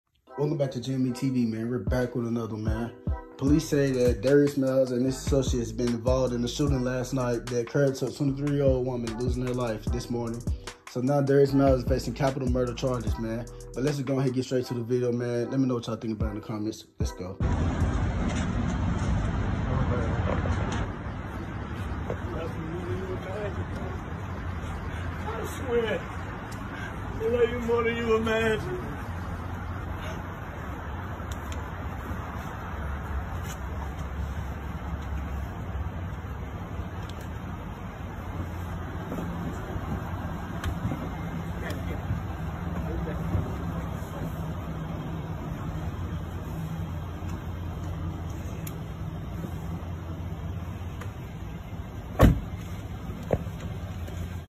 Alabama State Basketball Player Darius Miles Arrested For Capital Murder Today!
Add Us On FB GMETV📱
Add Us On TikTok GMETV📹
And Share This To Your Friends🌎